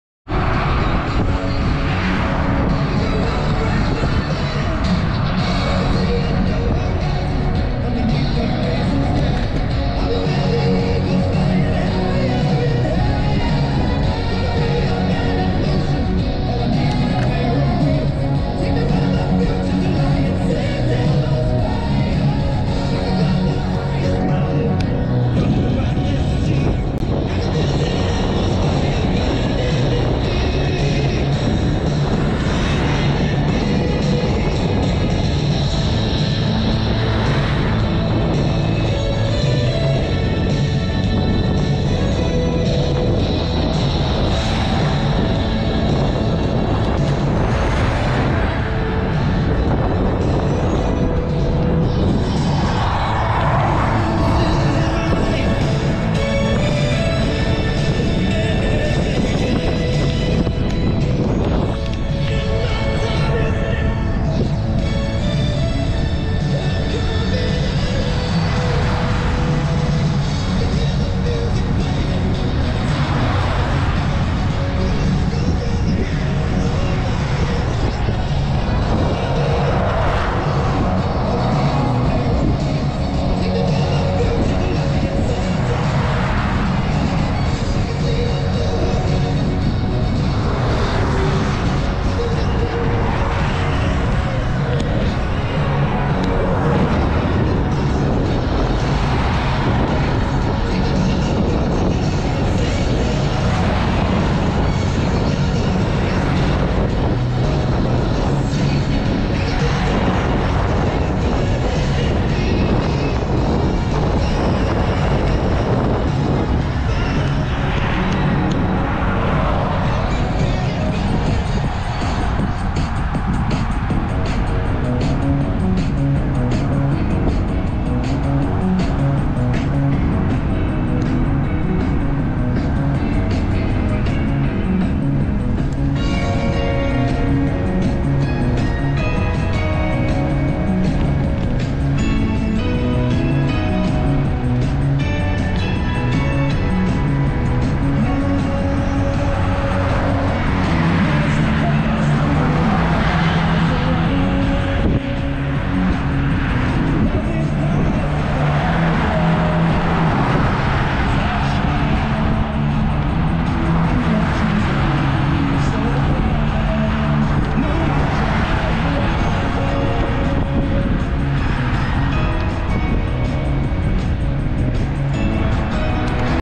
Go Pro Hero 8 Black Handlebar mount on bike
Ride nortbound on Petaluma Hill road  in Santa Rosa 360 pan around testing my new Go Pro Hero Black in the 4K setting .